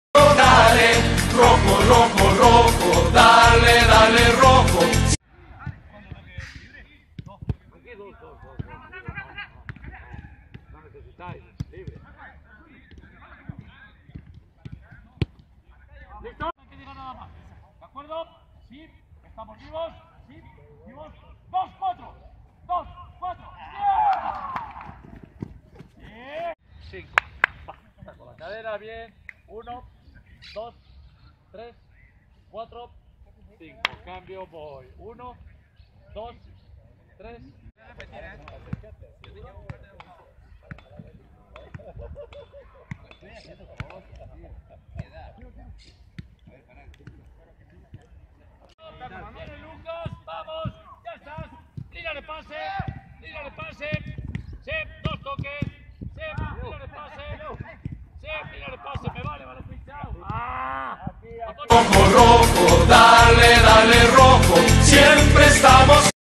Entrenamiento de Wilstermann en Argentina | Copa Libertadores 2019
El Rojo prepara el encuentro ante Boca Juniors